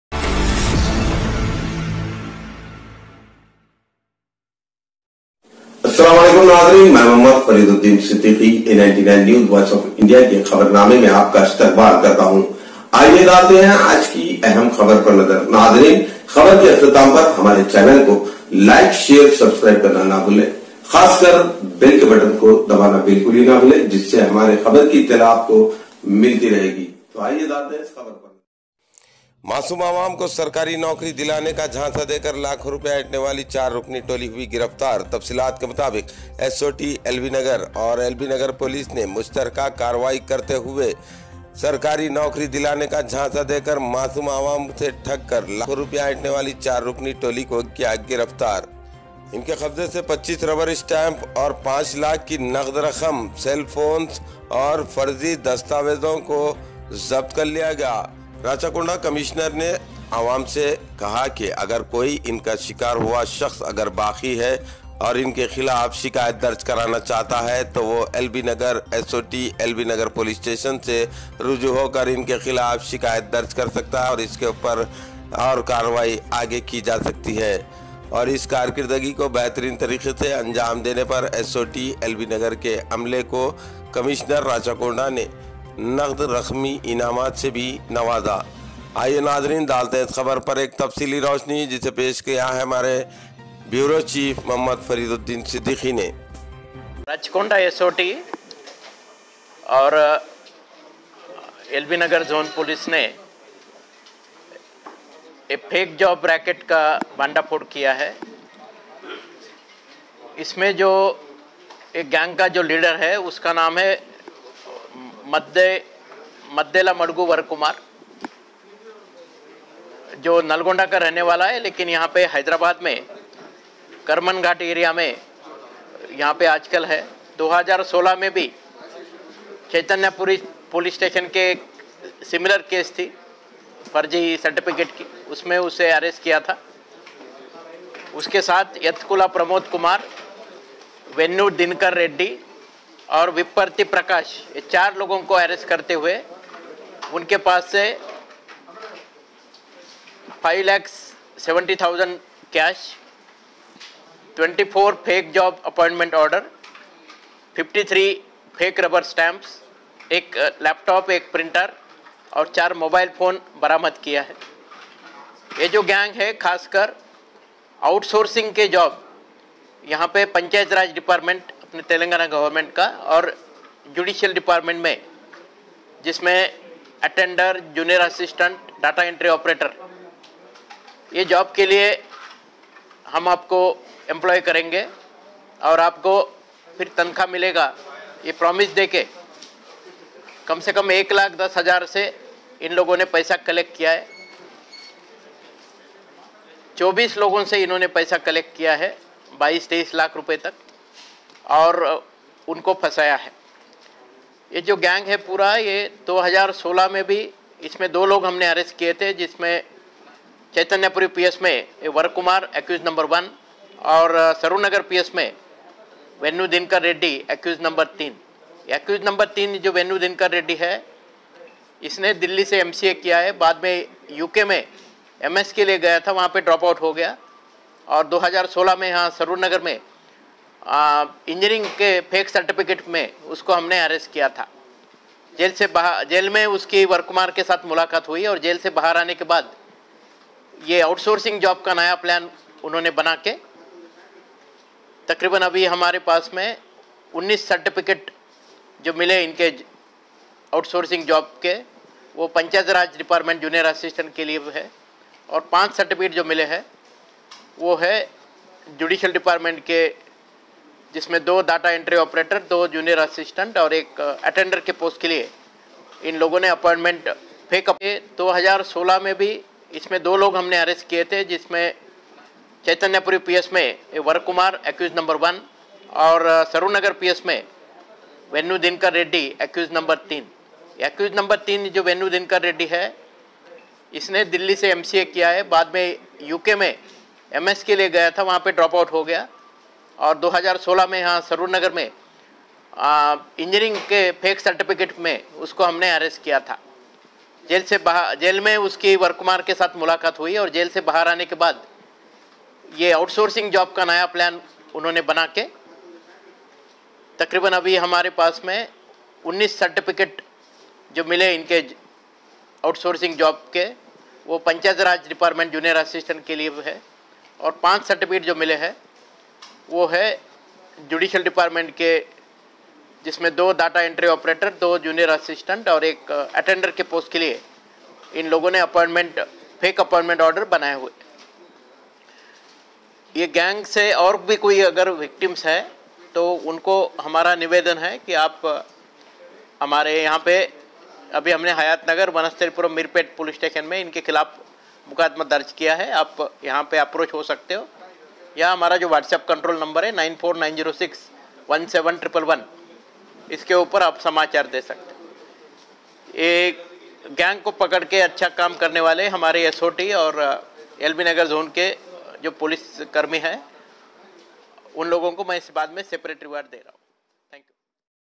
Sarkari Noukri Dilane ka jhansa dekar aawam lakho rupee ka gaban
SOT RACHAKONDA BUSTED THE FAKE JOB RACKET - ARRESTED FOUR NOTORIOUS CRIMINALS WHO CHEATED THE UN-EMPLOYED YOUTH UNDER THE GUISE OF PROVIDING OUTSOURCING/CONTRACT JOBS IN GOVT. SECTORS AND COLLECTD HUGE AMOUNTS IN LAKHS BY FURNISHING FAKE AND FORGED APPOINTMENT ORDERS - SEIZED NET CASH RS.5,70,000/-, (24) FAKE JOB APPOINTMENT ORDERS, (53) FAKE RUBBER STAMPS, (1) HP LAPTOP, (1) HP PRINTER AND (4) MOBILE PHONES FROM THEIR POSSESSION.
On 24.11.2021 at morning hours, on receipt of credible information, the sleuths of SOT LB Nagar zone of Rachakonda Commissionerate arrested (4) notorious criminals viz., 1) Maddelamadugu Varakumar, 2) Yathakula pramod Kumar, 3) Vennu Dinakar Reddy and 4) Vipparthy Prakash who cheated the gullible un-employed youth under the guise of providing jobs under outsourcing/ contract basis in Govt., sectors like judiciary & panchayathraj departments etc., and collected huge amounts in lakhs from them by furnishing fake and forged appointment order copies. Seized net cash Rs.5,70,000/-, (24) fake & forged appointment orders prepared by the accused persons, (53) Fake rubber stamps, (1) HP Laptop, (1) HP Printer and (4) mobile phones from their possession.
Cr.No.879/2021 U/s 120-B, 420, 406, 468, 473 ipc of hayathnagar ps,
Cr.No.985/2021 U/s  120-b,420,406,468, 473 ipc of vanasthalipuram ps,
Cr.No.931/2021 U/s 120-b, 420, 406, 468, 473 IPC of meerpet ps,                                          of rachaKONDA POLICE COMMISSIONERATE
DETAILS OF ACCUSED PERSONS:

Maddelamadugu Varakumar @ Varun s/o Anandam, Age: 36 years, Occ: Business, R/o Plot No.51, Sai Enclaves, Kharmanghat, Hyderabad, N/o Angadipet village, Pedda Adiserlapally mandal, of Nalgonda district.

Yathakula pramod Kumar s/o Babu, Age: 32 years, Occ: Business, R/o: Shivaganga colony, LB Nagar, Hyderabad.

Vennu Dinakar Reddy @ Dinesh Reddy @ Dinna s/o Ranga Reddy, Age: 44 years, Occ: Business, R/o: H.No.8-2-55/1, New Maruthi Nagar, Champapet, Kharmanghat, Hyderabad.

Vipparthy Prakash s/o Prasad Rao, Age: 50 years, Occ: Seals & stamps maker, R/o Balajinagar colony, B.N Reddy Nagar, Vanasthalipuram.

SEIZURES :
Net Cash Rs.5,70,000/- (Rupees Five Lakhs and Seventy thousand only).

(5) Fake & forged appointment orders said to have been issued by the judiciary dept., for the posts of Junior Assistant, Data Entry Operator, Attender

(19) Fake & forged appointment order said to have been issued by the Panchayathraj department for the posts of Junior Assistant,

(53) fake rubber Stamps of various offices and designators,

HP Company Laptop

HP Company Printer,
Mobile phones – (4),
BRIEF FACTS:

 The A-1 Maddelamadugu Varakumar @ Varun and A-2 Yathakula pramod Kumar are close friends and done real estate business. The A-3 Vennu Dinakar Reddy @ Dinesh Reddy @ Dinnu also did real estate business earlier, whereas the A-4 Vipparthy Prakash is a rubber stamps maker. The A-1 and A-3 are having past criminal history.  Of them, the A-1 indulged in similar offence i.e. fake job racket vide Cr.No.320/2016 U/s 420, 406, 466, 468, 471, 473, 120-B IPC of PS Chaithanyapuri, while the A-3 indulged in preparation of fake engineering certificates vide Cr.No.591/2016 U/s 420, 468, 471 r/w 34 IPC of PS Saroornagar.  In the above cases, the A-1 & A-3 were arrested and lodged in central prison Cherlapally at the same period.  As such, both of them got acquainted to each other and subsequently became close friends. During their tenure in the jail, they planned to work together to earn easy money by cheating the gullible people.

  Accordingly, after releasing from jail on bail, the A-1 & A-3 joined the A-2 Pramod Kumar with them. Of them, the A-1 completed the graduation, whereas the A-3 completed the MCA in New Delhi and discontinued the MS in UK.  Thus, the A-3 Vennu Dinakar Reddy has well computer knowledge and he is expert in designing the fake certificates of various universities/autonomous institutions.  As such, the A-1 asked the A-3 to prepare the fake appointment orders mentioning some G.O numbers that would appear to be similar to the original orders being issued by the concerned authorities. For execution of their plan, the A-1 to A-3 approached the A-4 who is professional in making the rubber stamps of different authorities.  In fact, the A-4 generally charges Rs.200/- to 300/- per each stamp, but when the A-1 to A-3 offered him huge amount i.e. Rs.1000/- per each stamp, he too willfully agreed for making the stamps of various authorities illegally.

  Thus, as stated by the A-1 to A-3, the A-4 has prepared nearly (17) round seals, (31) designated rubber stamps & (5) signature stamps of different offices and authorities of the State/Central Governments including Panchayathraj, judiciary, TS transco, TS Genco, Health, Irrigation, Metro & Railways etc., and handed over the same to the A-1 to A-3.  Thereafter, the A-1 to A-3 consulted one Khasim who has been running a job consultancy styled as ‘LEMINI CONSULTANCY’ in Kharmanghat area for